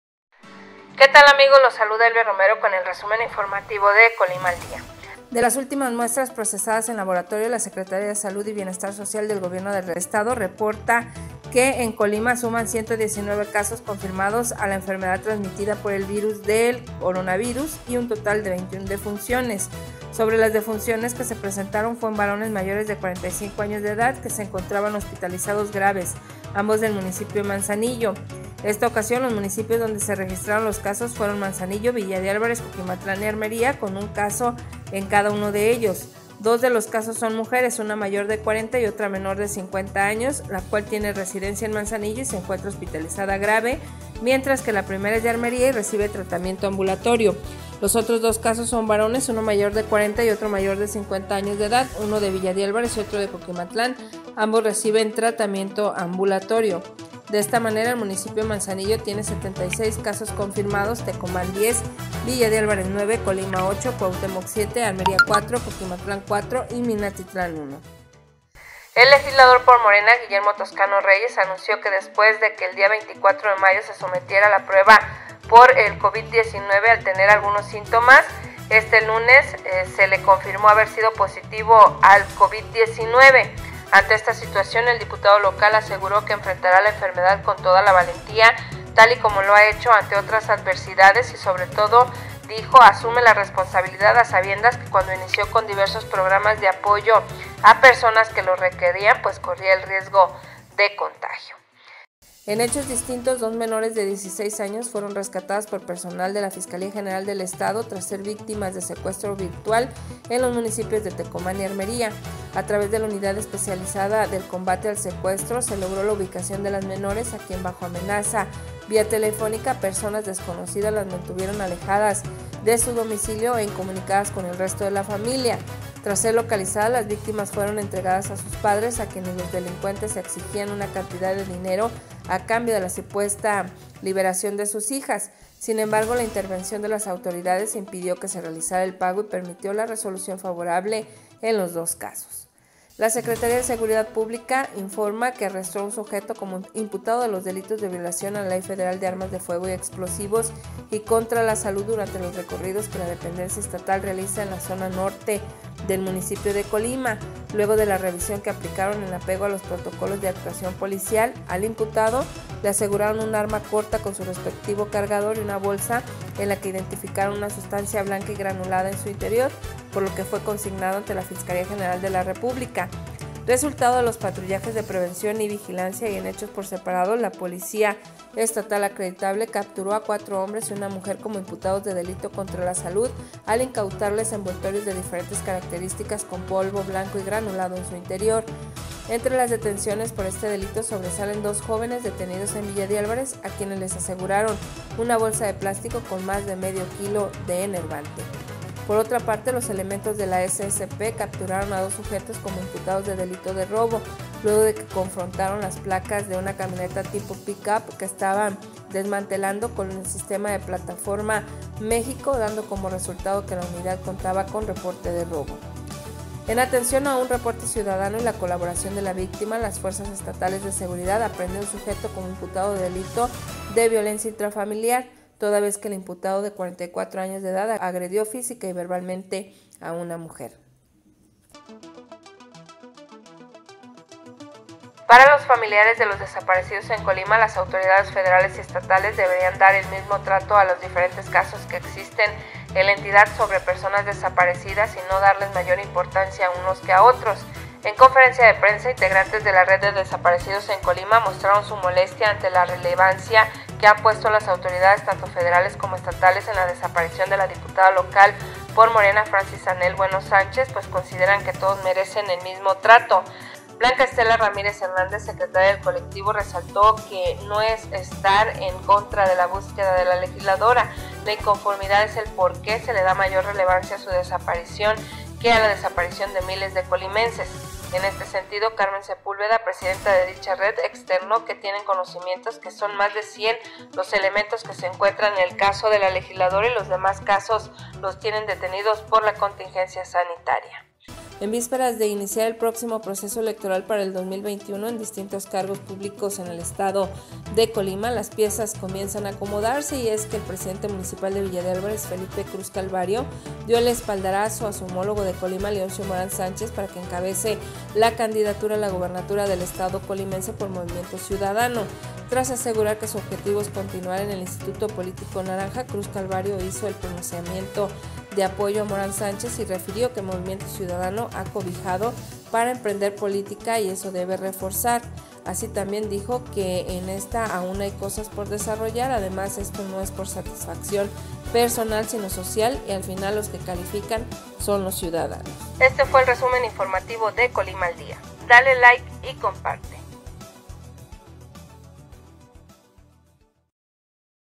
con Elvia Romero
Colima, suman 119 casos confirmados a la enfermedad transmitida por el virus del SARS-CoV-2, y un total de 21 defunciones, el legislador por Morena, Guillermo Toscano Reyes da positivo en COVID 19, dos menores de 16 años fueron rescatadas de ser víctimas de secuestro virtual, no hay desaparecidos de primera o segunda en Colima, increpan familiares de desaparecidos estas y más noticias en el ResumenInformativo Colima con Elvia Romero